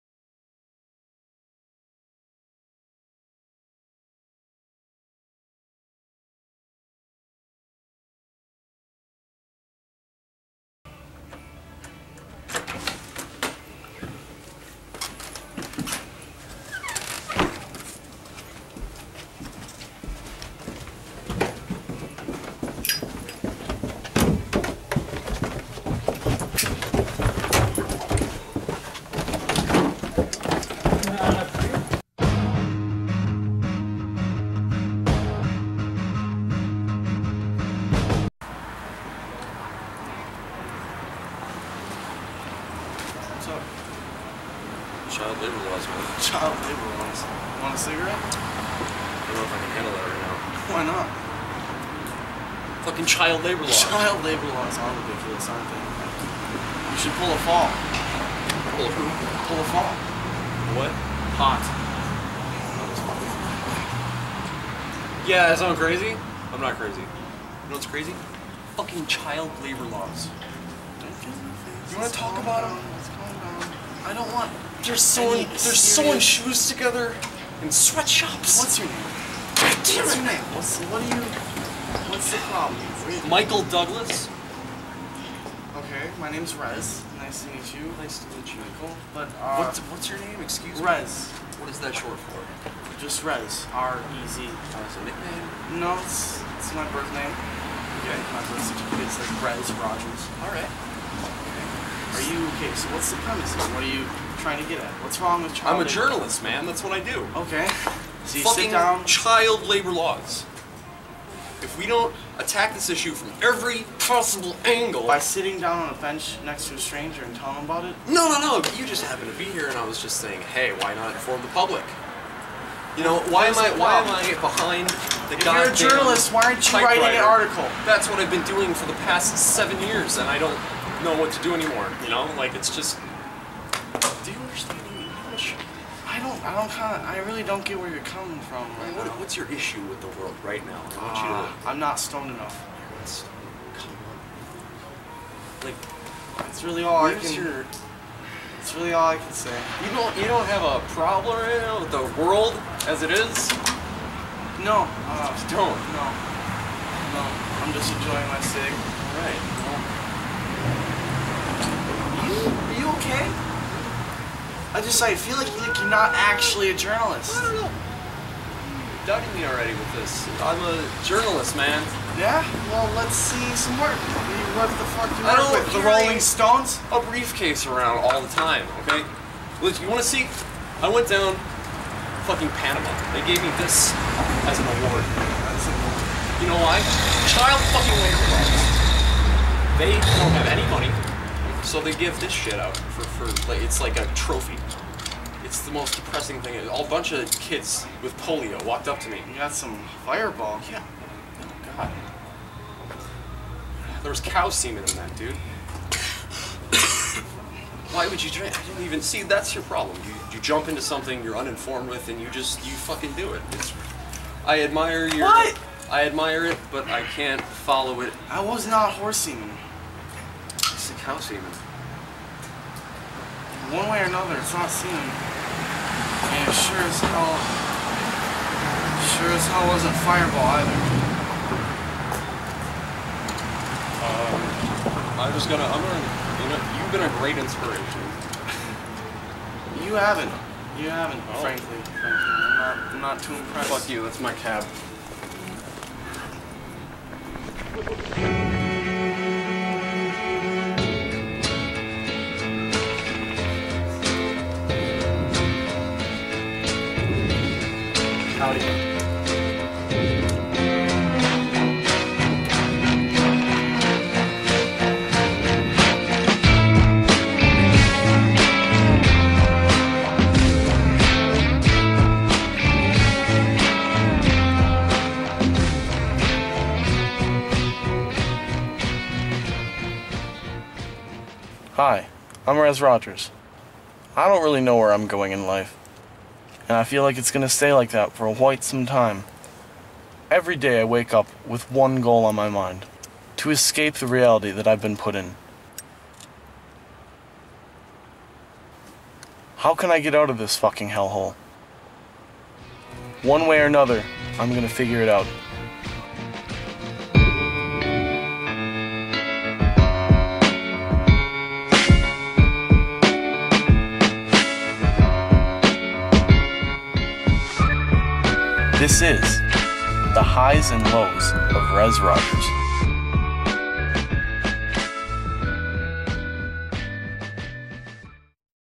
The Highs and Lows of Rez Rodgers - Series Preview
A laid-back 18 year old wanders about his home town in search of  a  good time, and maybe even the true virtues of life. He and his friends face a variety of taboo ordeals and situations; typically involving parties and illegal activities. Buckle up. Rez Rodgers is alive.